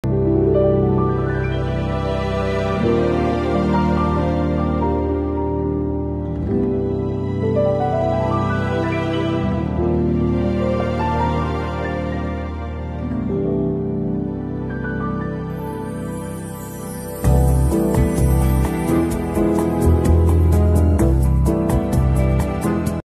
They set up a volleyball match in the airport. 😂🔥
Layovers call for creativity 😂 (via @brenopaulinelli/IG)